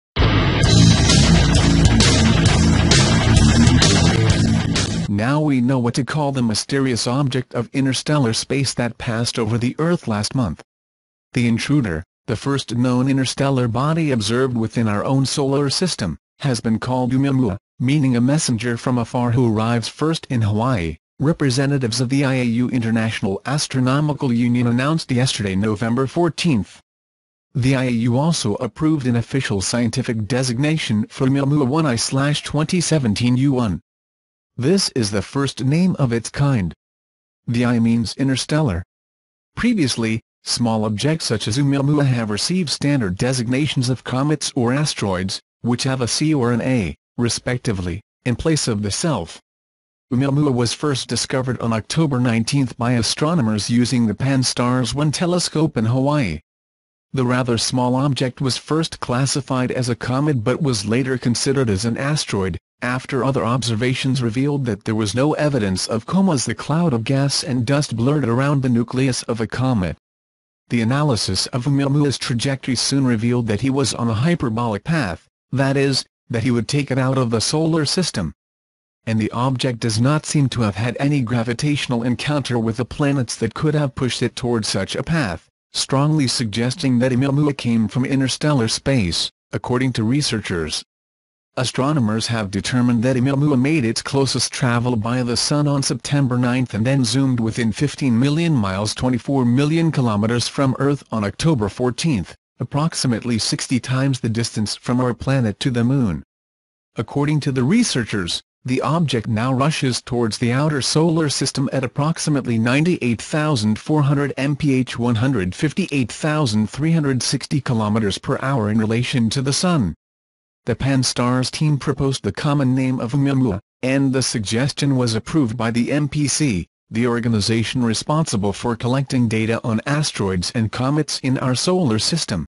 1Meet Oumuamua: The first interstellar visitor ever seen gets a name.
Meet Oumuamua: The first interstellar visitor ever seen gets a name.

Now we know what to call the mysterious object of interstellar space that passed over the Earth last month. The intruder - the first known interstellar body observed within our own solar system - has been called 'Oumuamua, meaning' a messenger from afar who arrives first 'in Hawaii, representatives of the IAU International Astronomical Union announced yesterday November 14th. The IAU also approved an official scientific designation for 'Oumuamua 1I / 2017 U1. This is the first name of its kind; the I means interstellar. Previously, small objects such as' Oumuamua have received standard designations of comets or asteroids, which have a C or an A, respectively, in place of the self.

Oumuamua was first discovered on October 19 by astronomers using the Pan-STARRS1 telescope in Hawaii. The rather small object was first classified as a comet but was later considered as an asteroid, after other observations revealed that there was no evidence of coma as the cloud of gas and dust blurred around the nucleus of a comet. The analysis of 'Oumuamua's trajectory soon revealed that he was on a hyperbolic path, that is, that he would take it out of the solar system. And the object does not seem to have had any gravitational encounter with the planets that could have pushed it towards such a path, strongly suggesting that 'Oumuamua came from interstellar space, according to researchers.

Astronomers have determined that 'Oumuamua made its closest travel by the sun on September 9 and then zoomed within 15 million miles 24 million kilometers from Earth on October 14, approximately 60 times the distance from our planet to the moon. According to the researchers, the object now rushes towards the outer solar system at approximately 98,400 mph 158,360 km / h in relation to the sun. The Pan-STARRS team proposed the common name of Oumuamua, and the suggestion was approved by the MPC, the organization responsible for collecting data on asteroids and comets in our solar system.